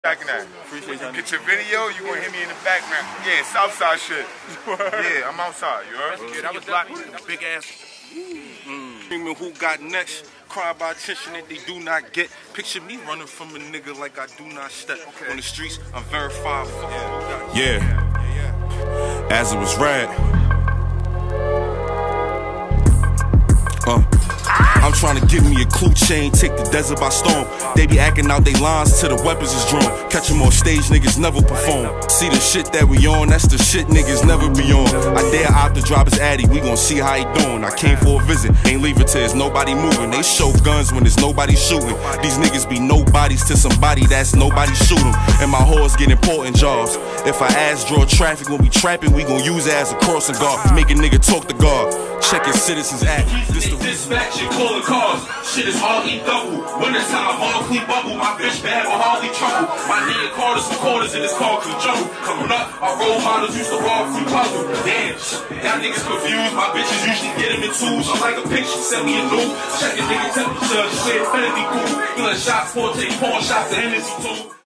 REDD PREZZI OPENS FOR LLOYD BANKS VLOG 1/28/24
REDD PREZZI was given the opportunity to open up for @LloydBanks at an event promoted by​⁠ @smashblocktv6903